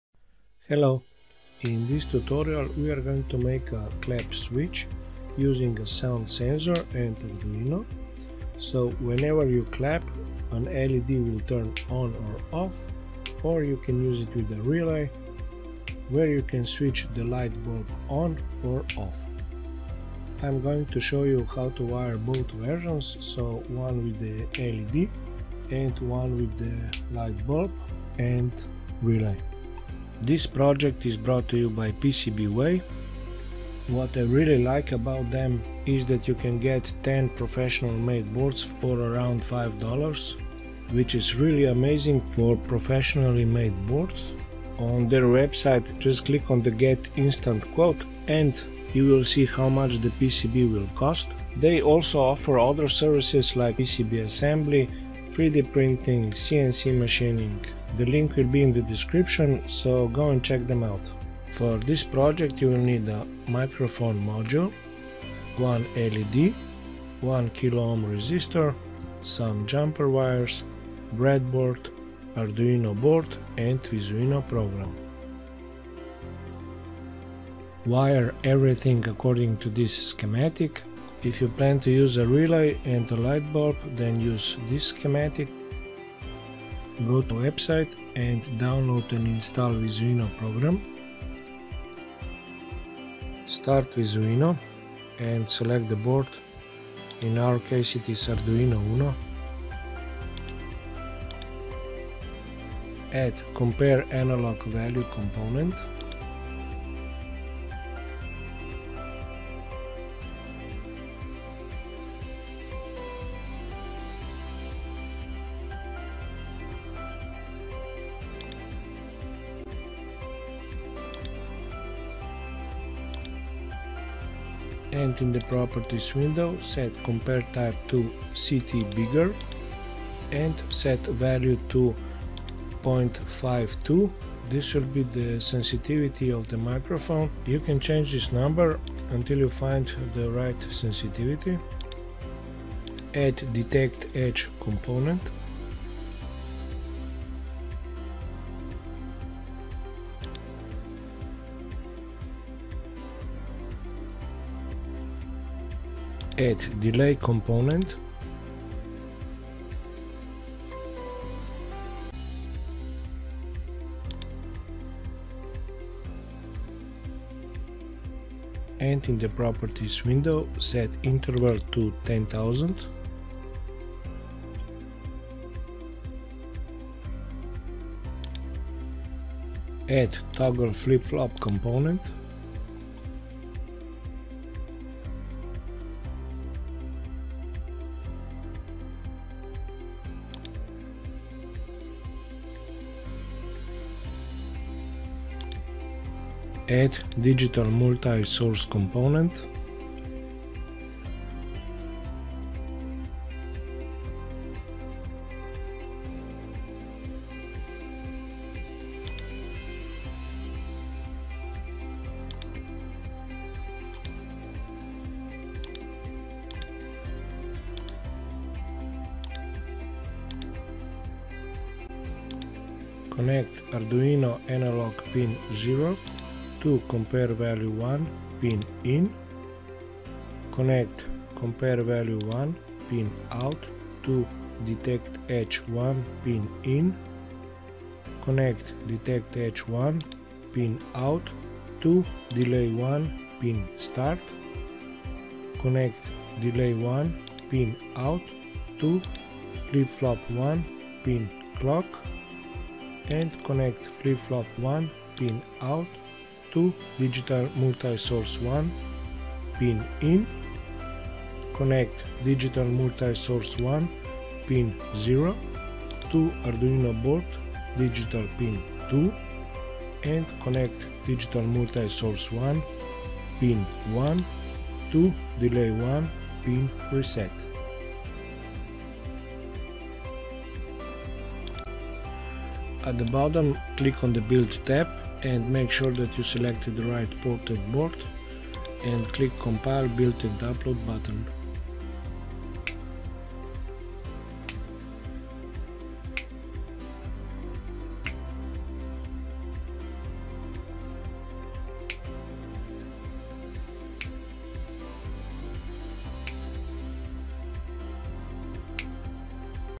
Clap Switch Using Sound Sensor & Arduino
In this tutorial we are going to make a clap switch using a Sound Sensor & Arduino. Whenever you clap an LED will Turn On or Off. There is also a schematic below on how to use it with a relay and a light bulb.